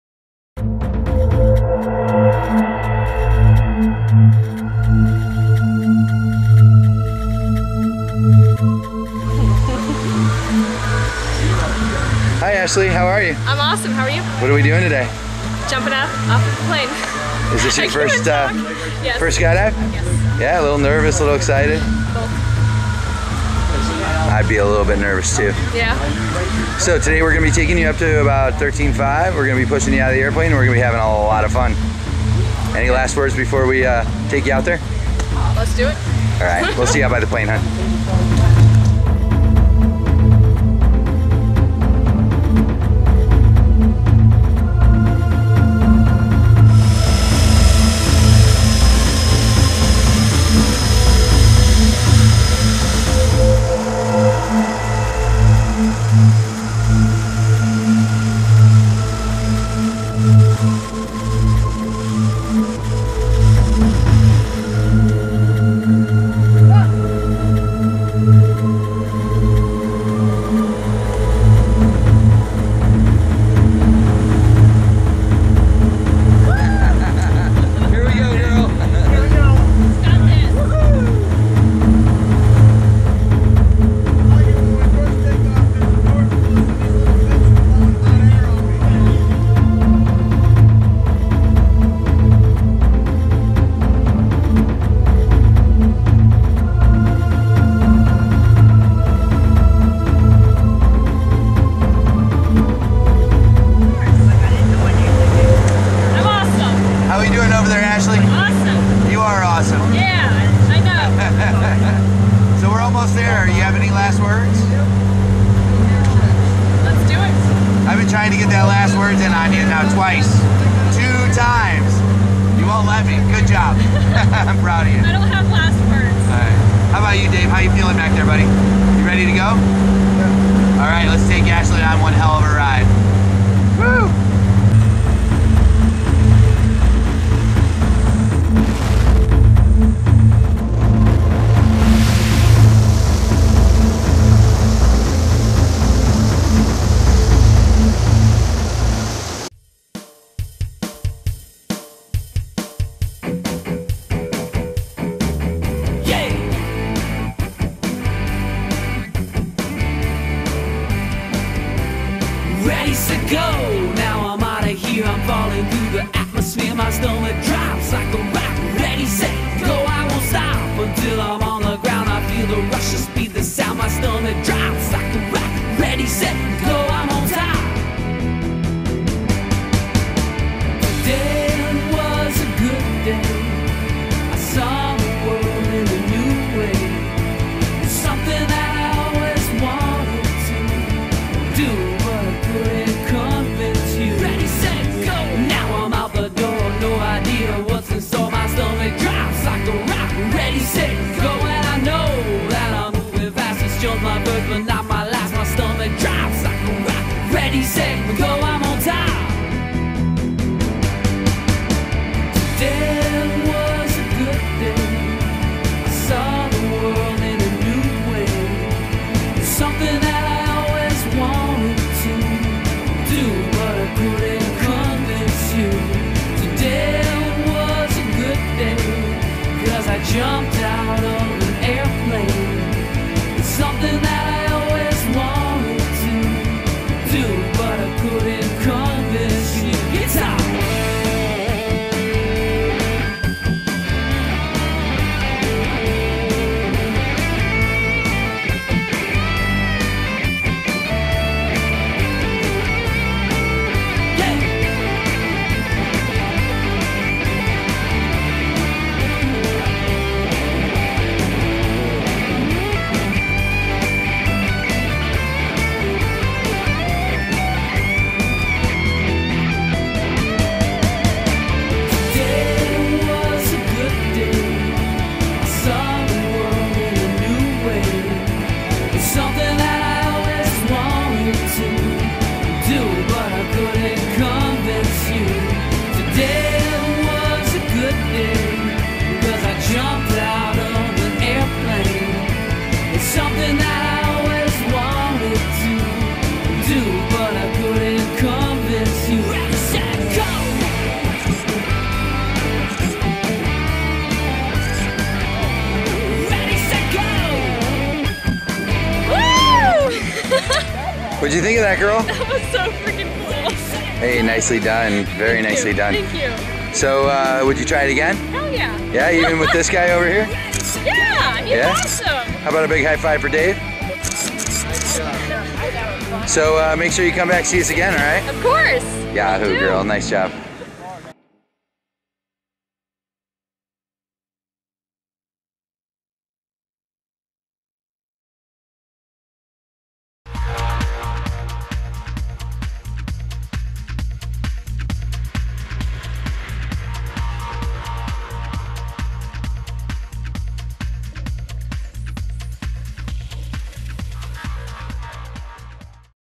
Ashley 7/21/12
Tandem skydive with Dave S.
Video by Aaron S.